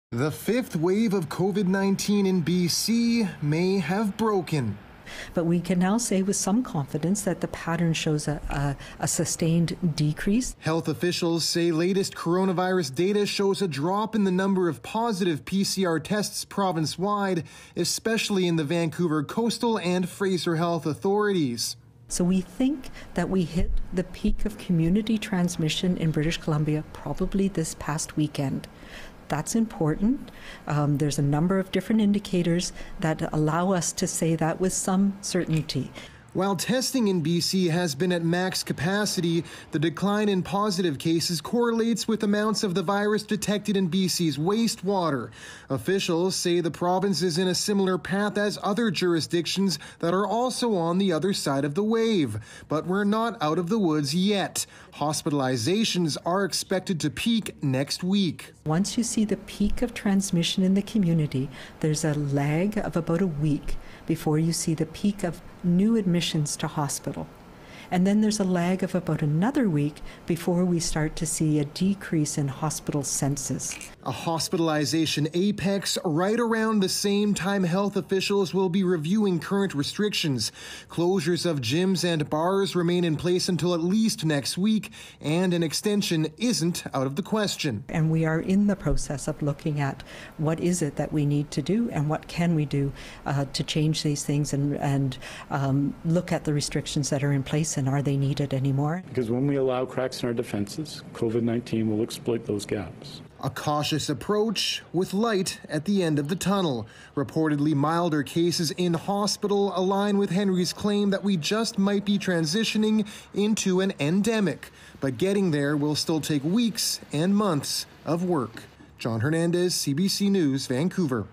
Omicron transmissions are in decline but hospitalizations could peak: modelling
Provincial Health Officer Dr. Bonnie Henry presented the latest modelling projections in a news conference Friday. She says the Omicron wave has spread faster than previous ones and health officials believe the top of the latest surge has already been reached.

The data shows while community transmission is trending down in most health regions, there is likely to be a rise in hospitalizations in the coming week or two.